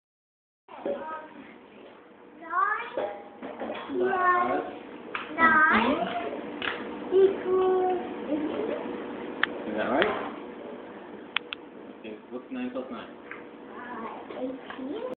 NIb talking to Anjum uncle about numbers
Nib and anjum